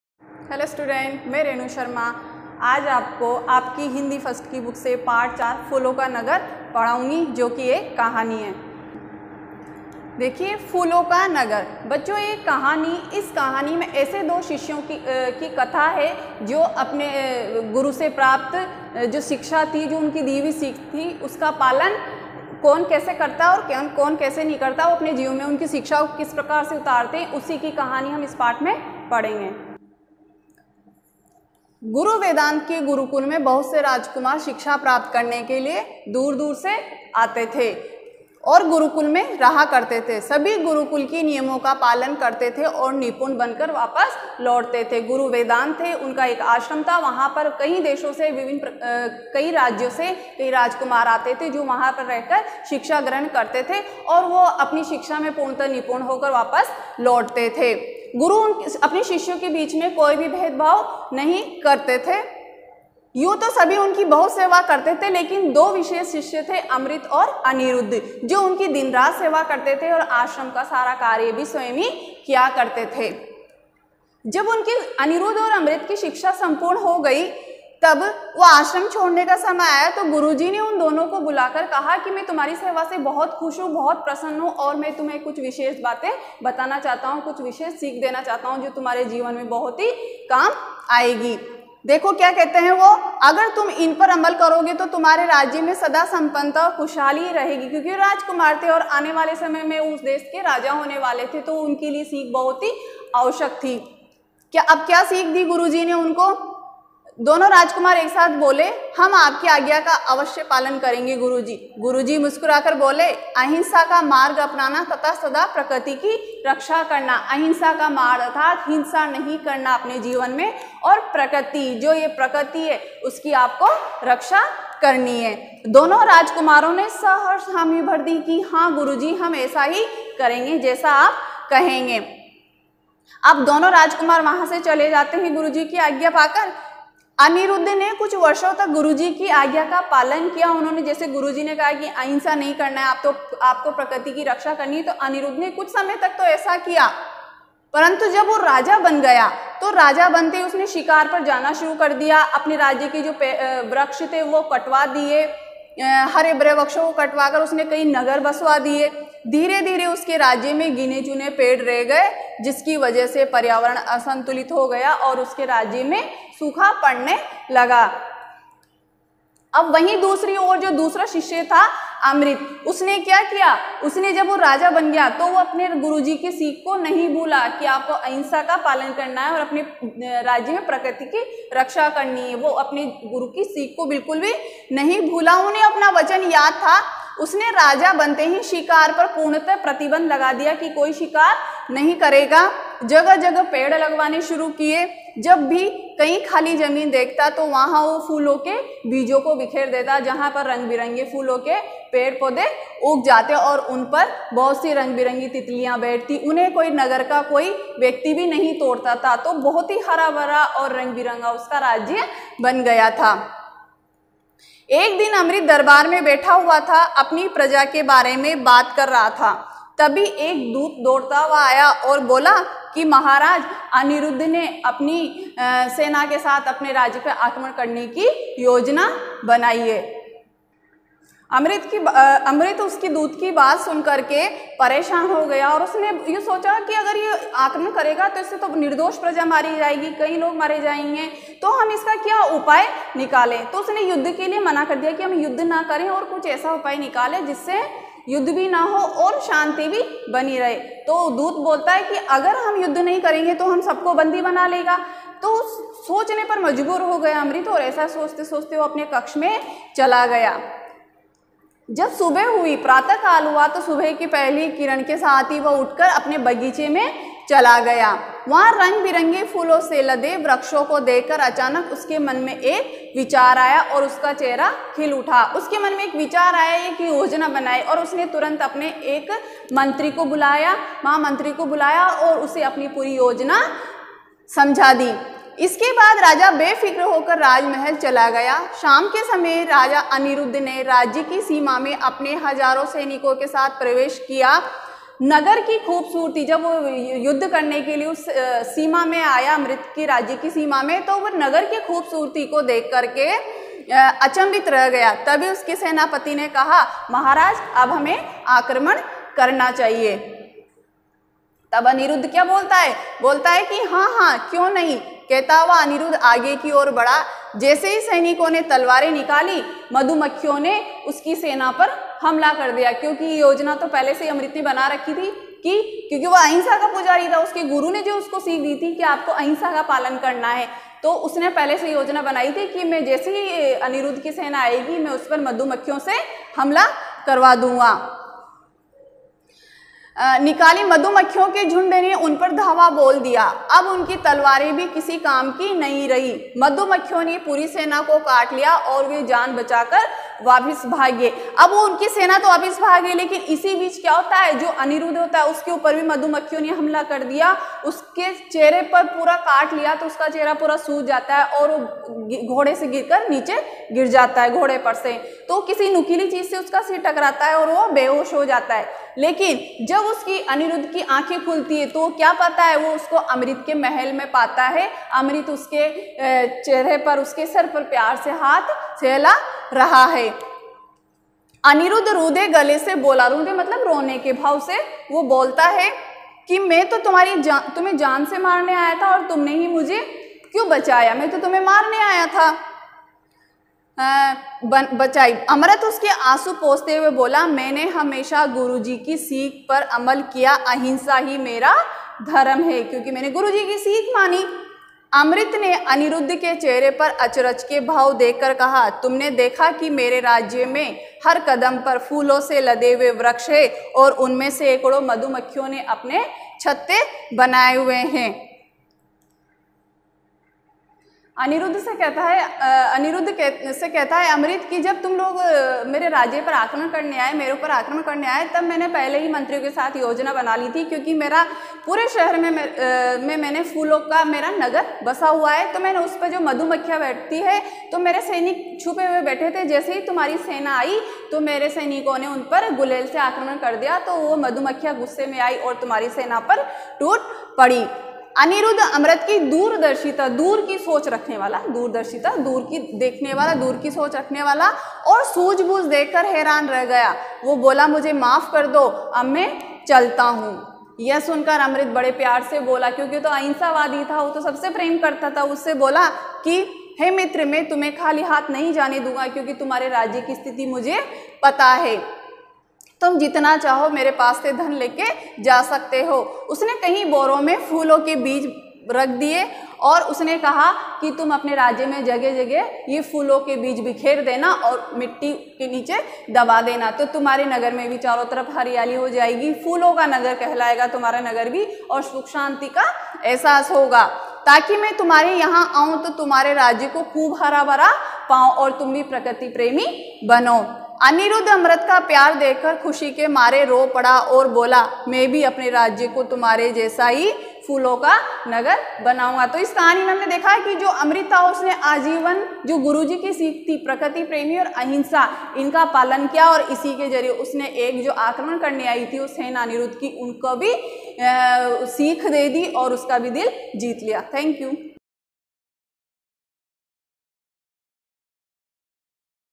RS_4th_Hindi Text_ Ch- 4_फूलों का नगर
Teacher- Mrs. Renu Sharma
Subject- Hindi Text
Class- 4th
 पाठ-4
फूलों का नगर